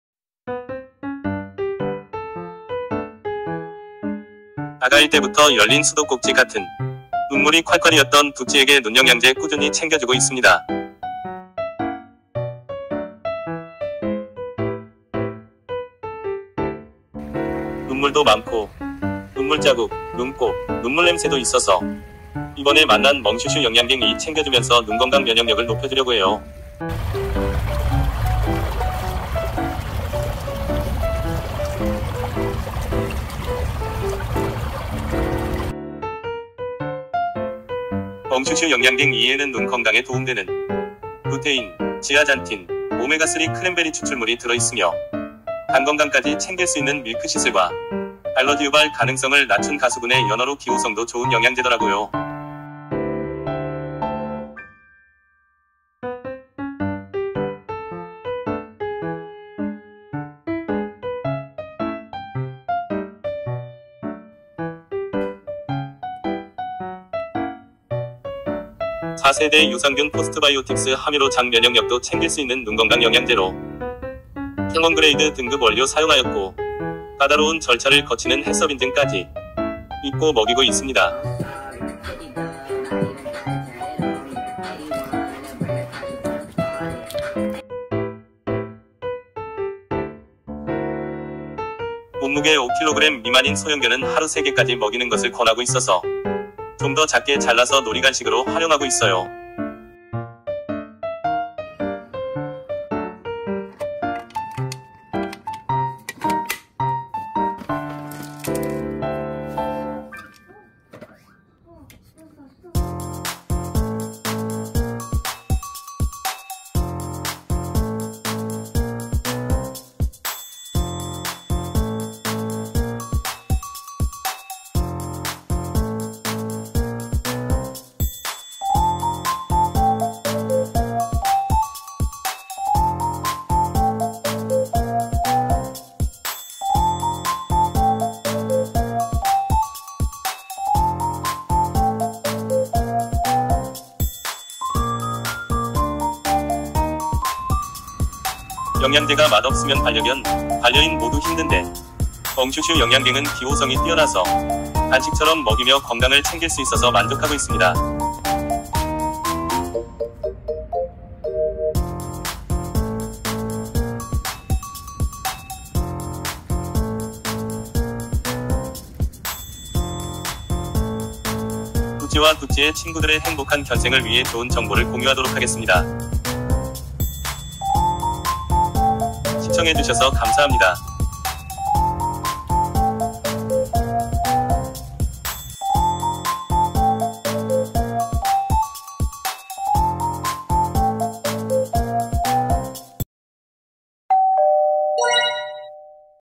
시츄일기74👍눈물 많은 시츄의 강아지눈영양제 간식처럼 맛있는 멍슈슈 영양갱E
눈물이 또르륵... 눈물자국, 눈물냄새, 눈꼽, 멍슈슈 영양갱E